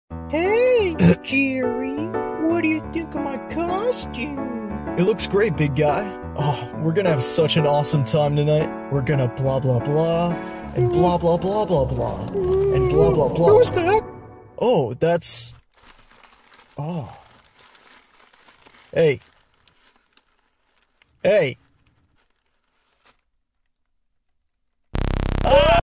spooky party
two ghost homies hold a spooky party

just a lil filler video for you guys as I work on some more ambitious projects ;)
thank you for supporting my goofy content! Happy Halloween!!